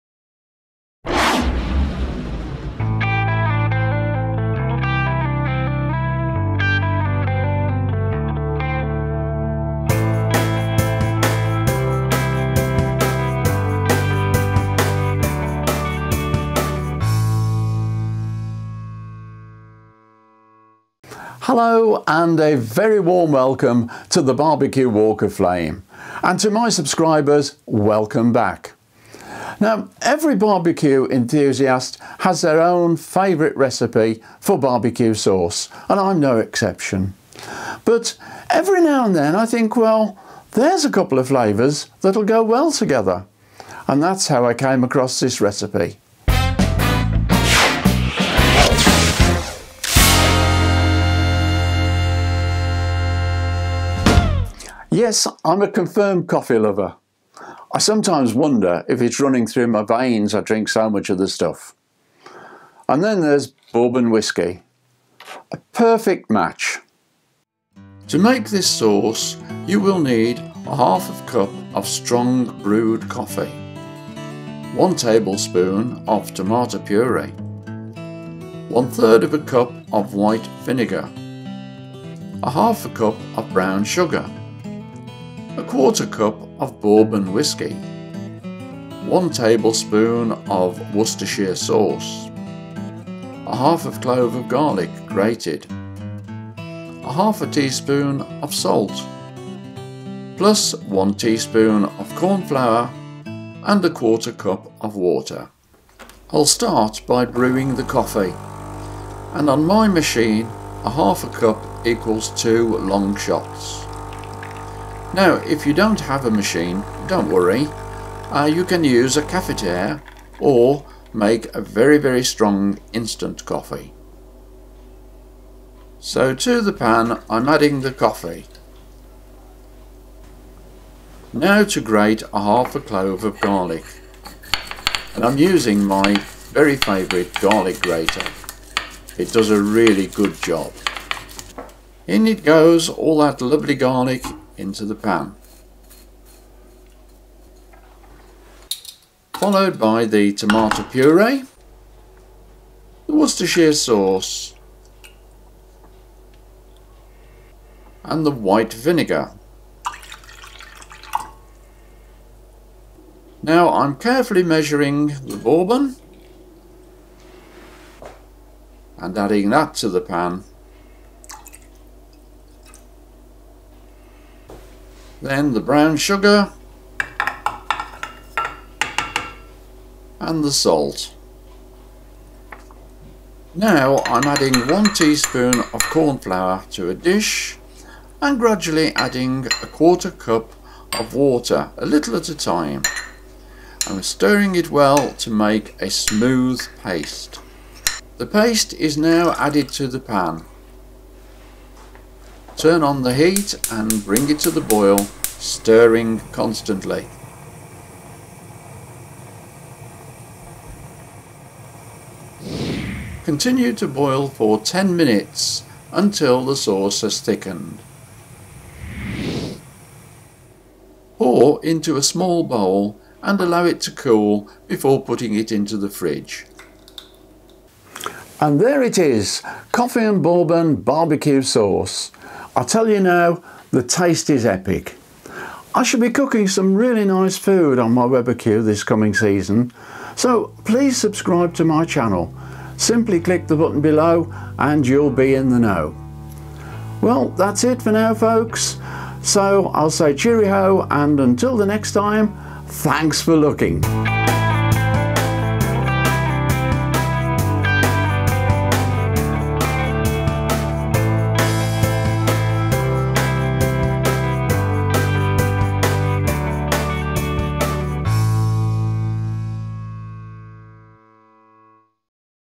Coffee & Bourbon Barbecue Sauce
See how I make this delicious barbecue sauce using two of my favourite ingredients, coffee, and bourbon whisky. An essential for any barbecue enthusiast, this quick and easy to make sauce goes well with burgers, and all grilled meats, also the taste is absolutely epic.